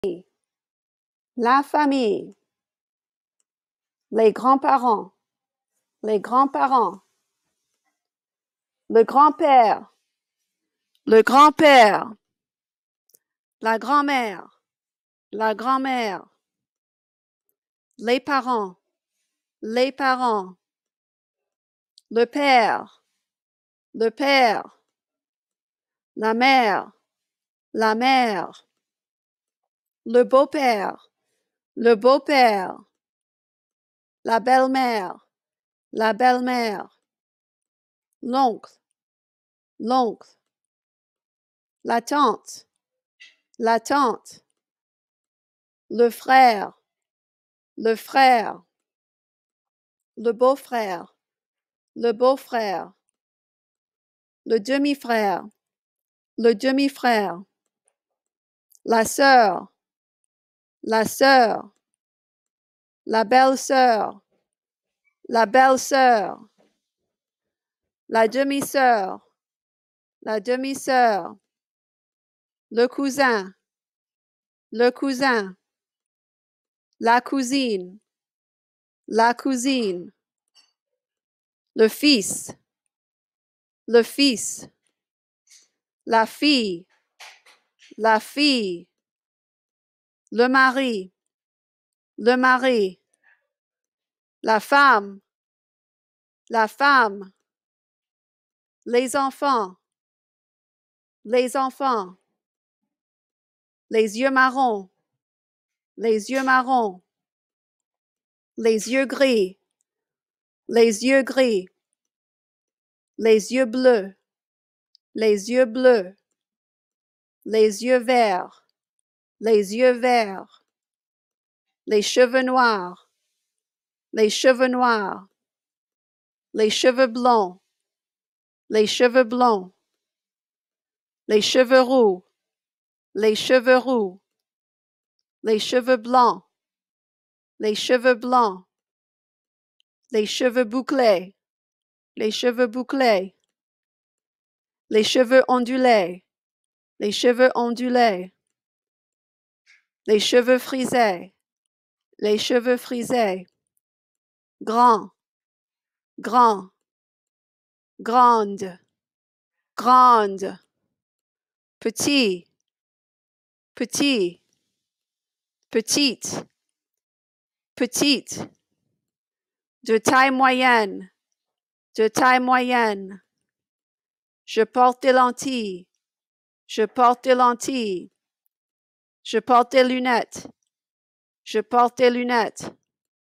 French Family Vocabulary
This is an introduction to Family Vocabulary. The vocabulary is in conjunction with unit 5A of the Level 1 T'es Branché series. Words are repeated twice in French.